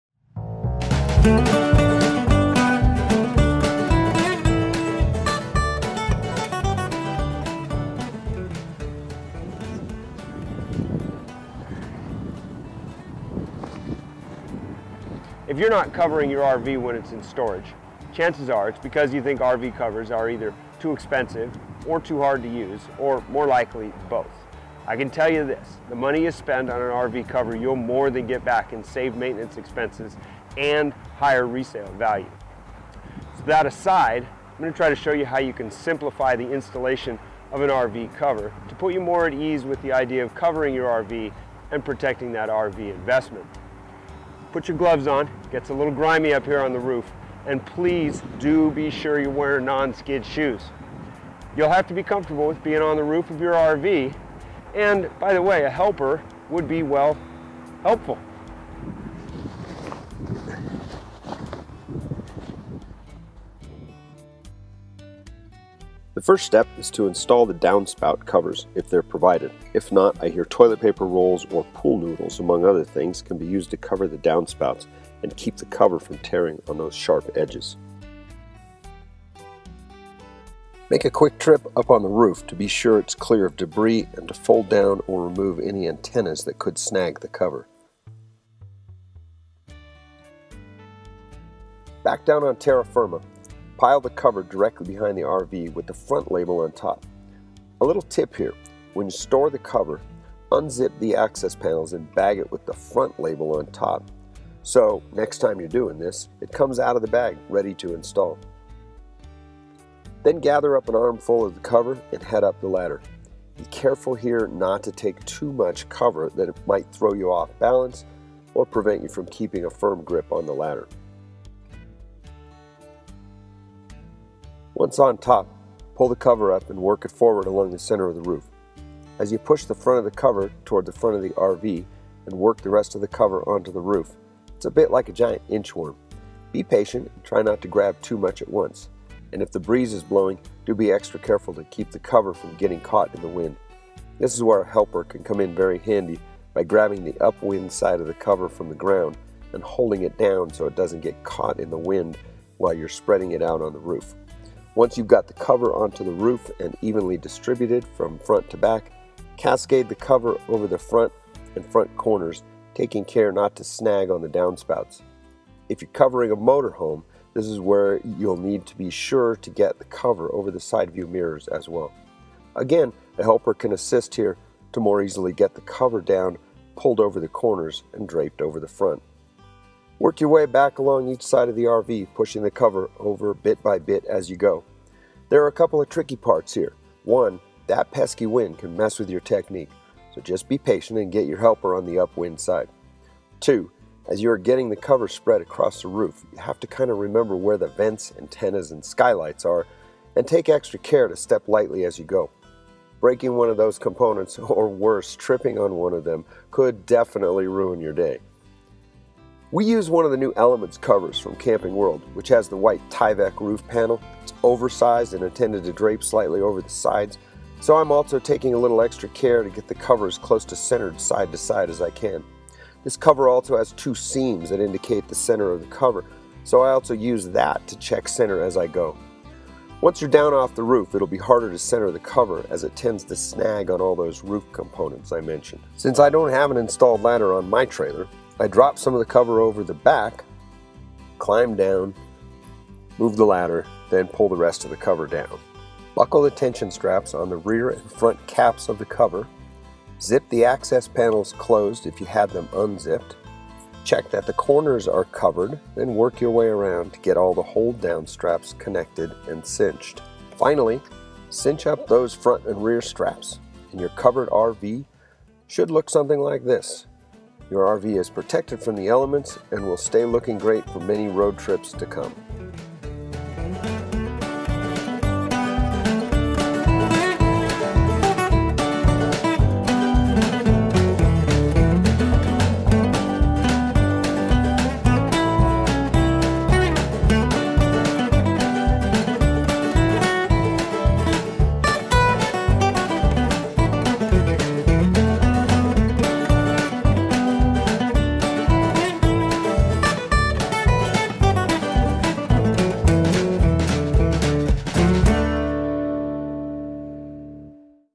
How to Cover Your RV
Trailer Life Magazine's Bob Dawson has a step-by-step tutorial on the installation of an RV cover.